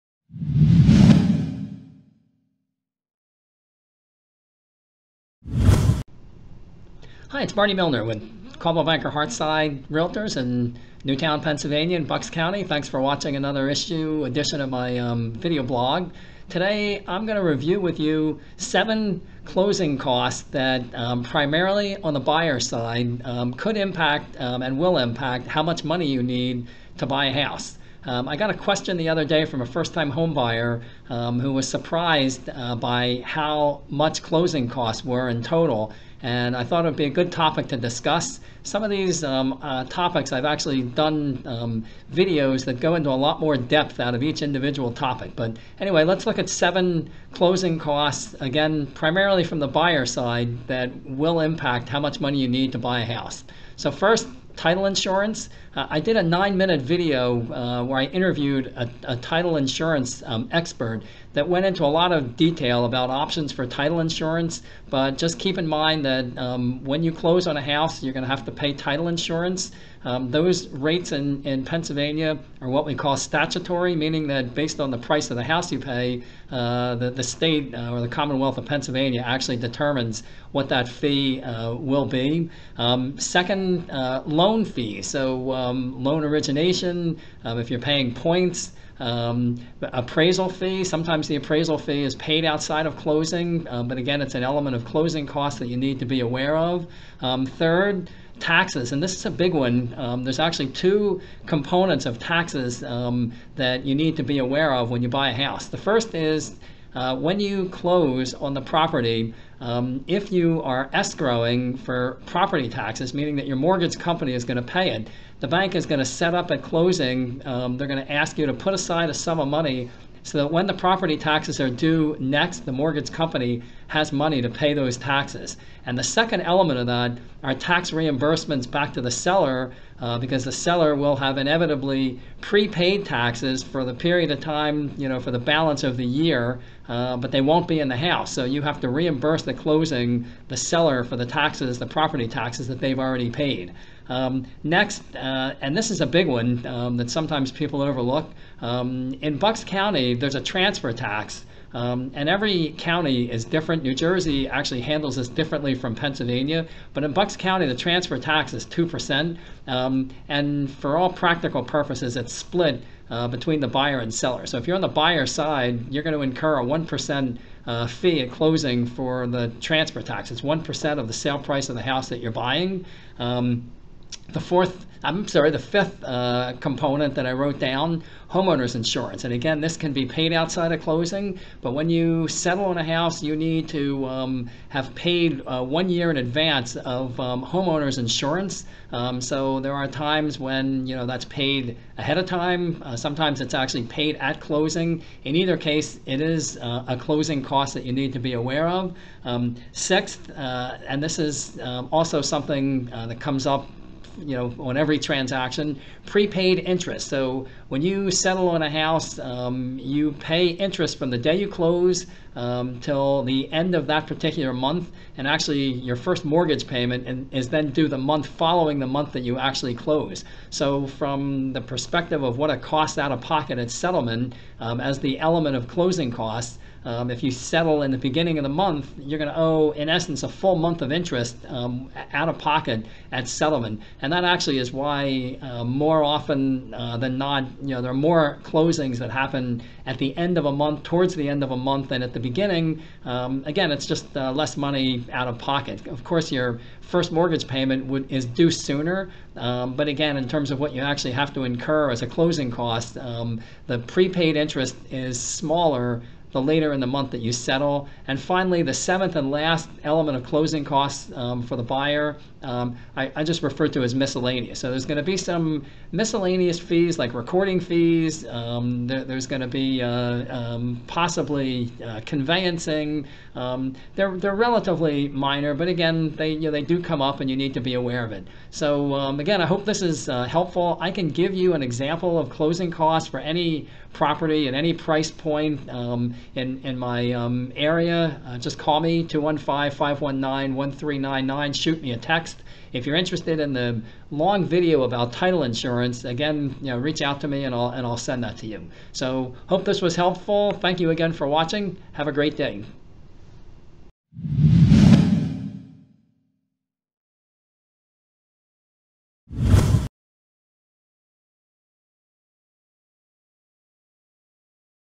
Bucks County Real Estate - Important closing costs to know
- Sometimes home buyers can be taken aback at how much they have to pay in closing costs. Today I wanted to talk about the seven most impactful closing costs every buyer needs to prepare for so that they don’t end up spending money they can’t afford to spend. The first of these fees is the title insurance. Rates in Pennsylvania are what we call “statutory,” meaning the state determines them based on the price you pay for the house. To know all of the most important closing costs, watch this short video.

Buying a home?

Selling a home?

Martin Millner
Coldwell Banker Hearthside
70 W. Afton Ave Yardley, PA 19067